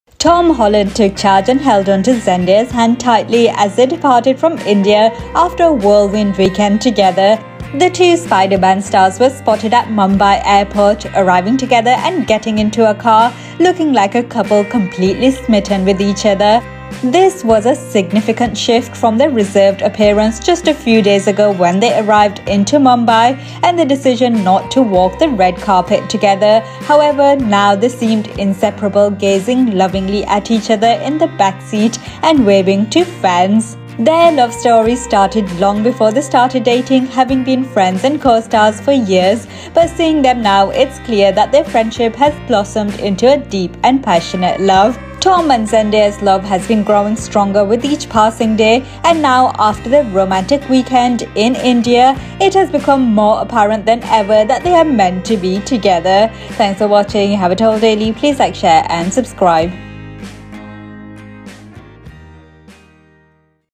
Tom Holland Holds Hands with Zendaya as They Leave Mumbai Together
Tom Holland took charge and held onto Zendaya's hand tightly as they departed from India after a whirlwind weekend together. The two Spider-Man stars were spotted at Mumbai airport, arriving together and getting into a car, looking like a couple completely smitten with each other.

This was a significant shift from their reserved appearance just a few days ago when they arrived into Mumbai and their decision not to walk the red carpet together. However, now, they seemed inseparable, gazing lovingly at each other in the backseat and waving to fans.

Their love story started long before they started dating, having been friends and co-stars for years. But, seeing them now, it's clear that their friendship has blossomed into a deep and passionate love.

Tom and Zendaya's love has been growing stronger with each passing day, and now, after their romantic weekend in India, it has become more apparent than ever that they are meant to be together.